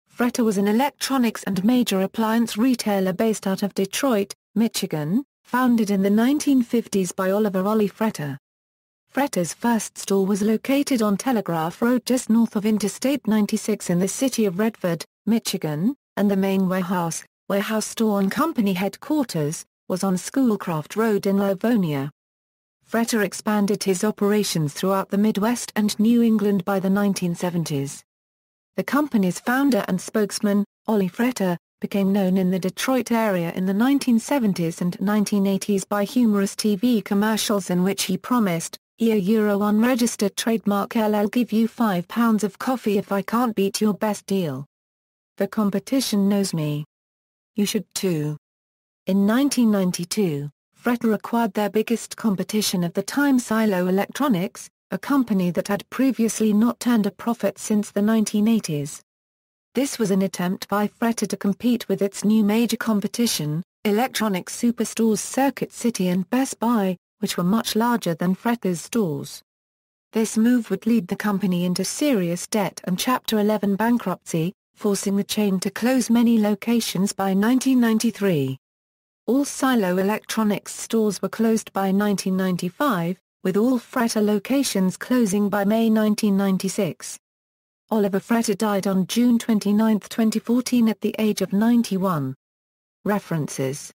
Fretter
Fretter was an electronics and major appliance retailer based out of Detroit, Michigan, founded in the 1950s by Oliver "Ollie" Fretter.
Fretter's first store was located on Telegraph Road just north of Interstate 96 in the city of Redford, Michigan, and the main warehouse, warehouse store and company headquarters, was on Schoolcraft Road (I-96 service drive) in Livonia. Fretter expanded his operations throughout the midwest and New England by the 1970s.